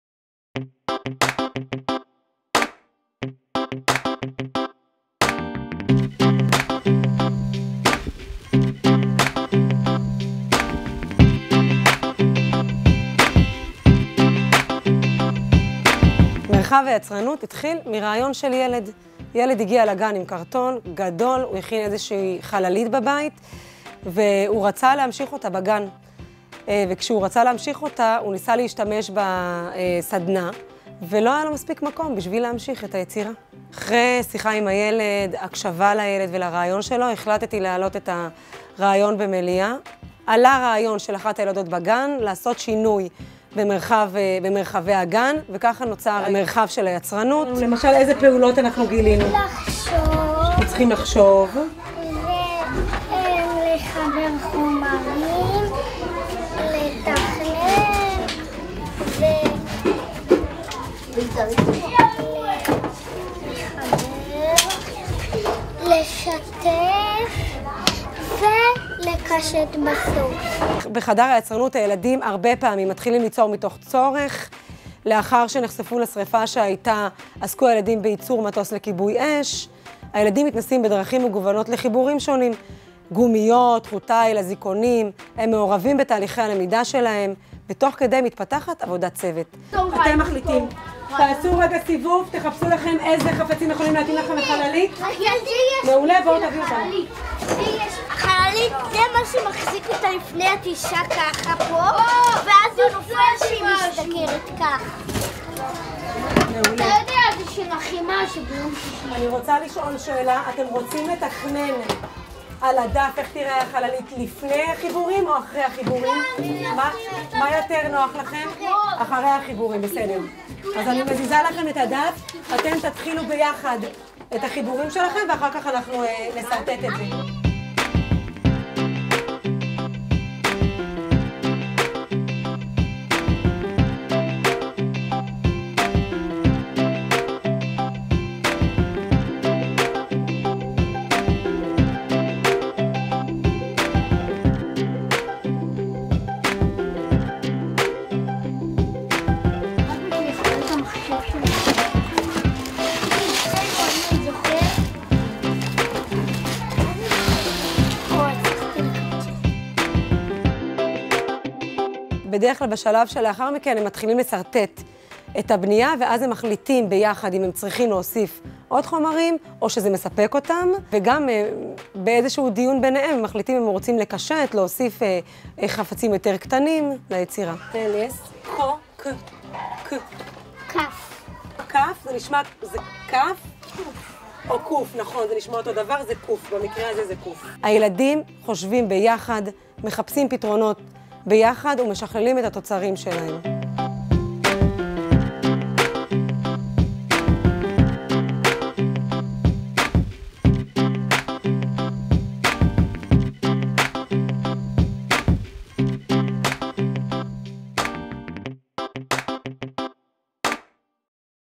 מרחב יצרנות בגן - מיוזמה לביצוע
איך נוצר מרחב יצרנות בגן?  הקשבה של תמי, מנהלת גן הדס, לצורך שעלה מאחד הילדים, הובילה לדיון במליאה והעלאת רעיונות שבעקבותיו הוקם בגן מרחב יצרנות. המרחב מאפשר לילדים להביא לידי ביטוי את יוזמותיהם תוך שכלול מיומנויות כגון:. עבודת צוות, עבודה עם חומרי חיבור שונים, כתיבה, תכנון ועוד.